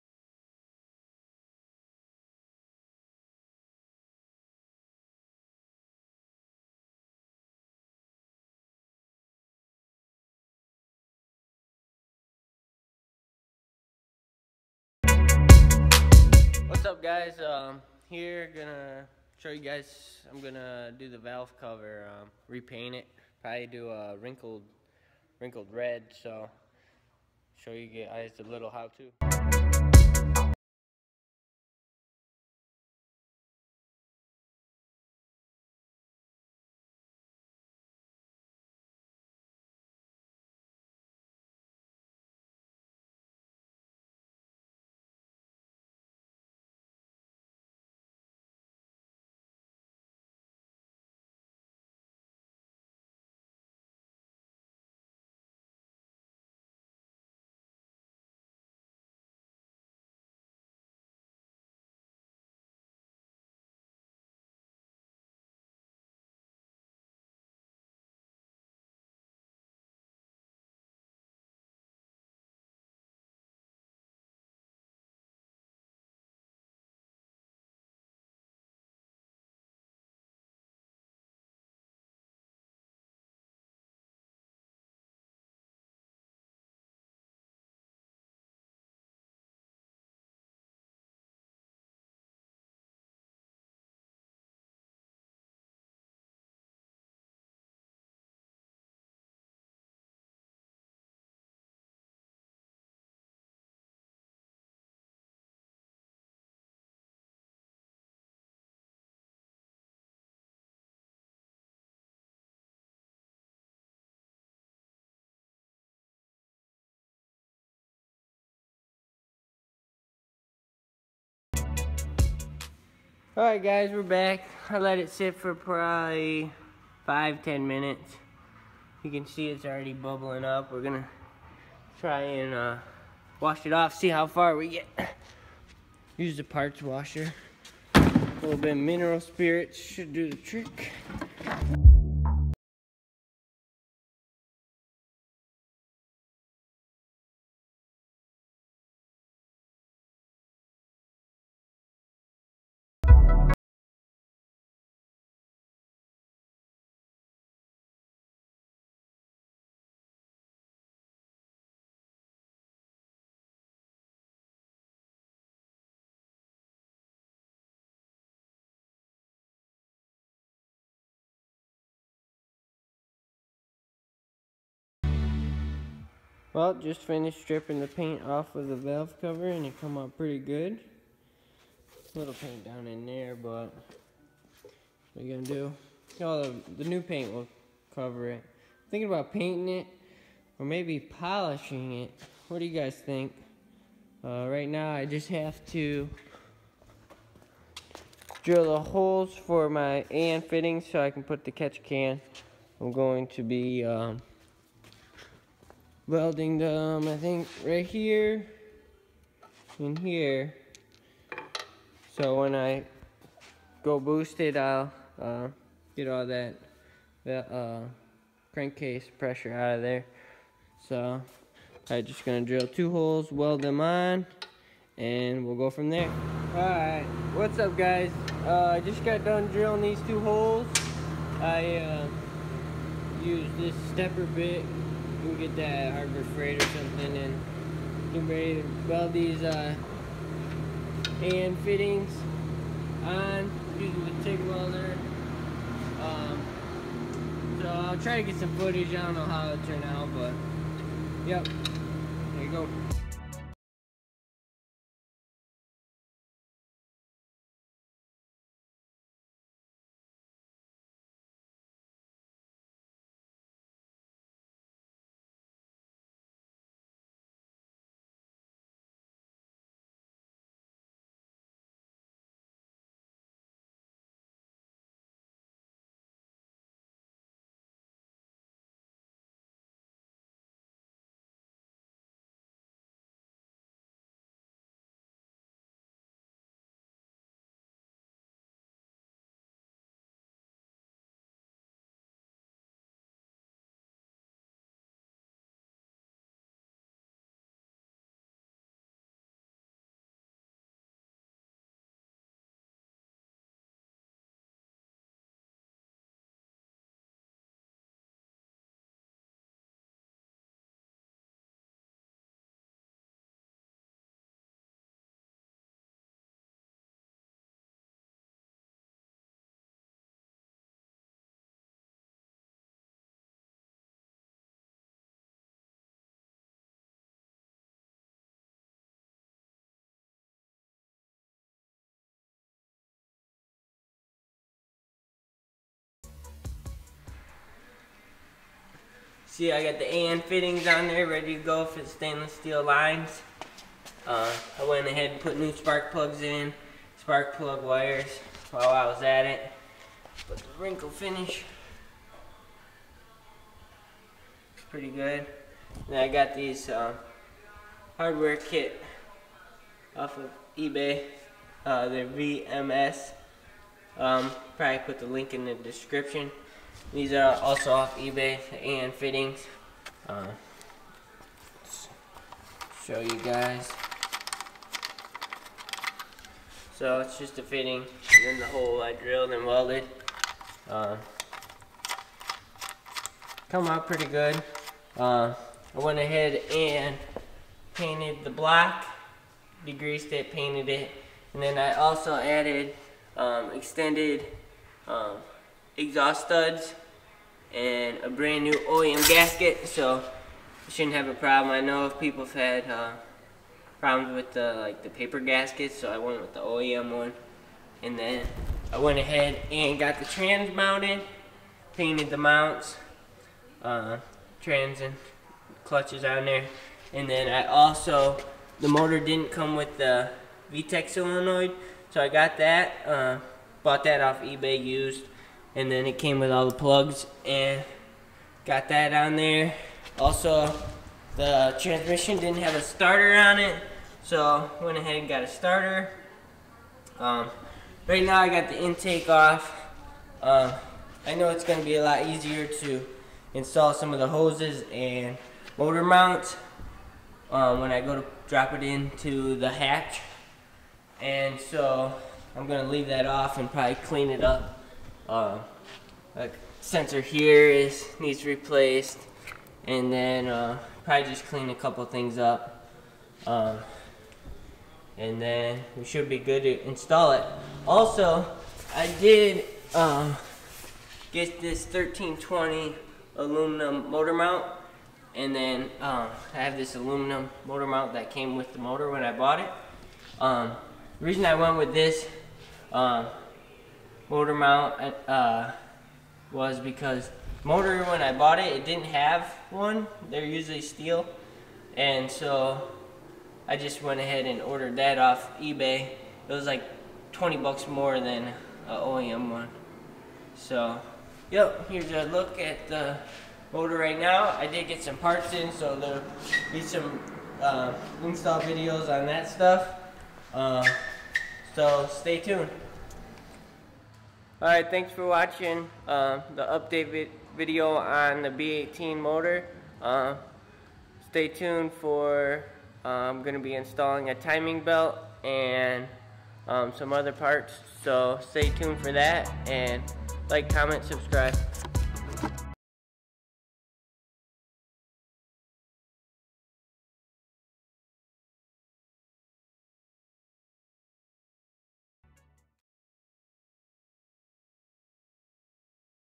Update: Valve cover Repaint and Added An fittings on New B18c1 motor
In this video I'm showing you how pull your b series Honda valve cover off, strip the paint off and paint it wrinkle red. Also adding an fittings for catch while it's off. It did take three coats of paint stripper before it all came off! Using water to rinse it off after each coat worked a lot better than mineral spirits.

Also gave you the first look at my new B18c1 motor that I will be putting in my Honda Civic EG hatch soon!

Links to the parts used

-10 an weld on fittings

-10an 90degree

Valve cover dress up kit

Spark plugs: ngk bkr7es

Oem exhaust gasket

Extended exhaust studs

1320 motor mount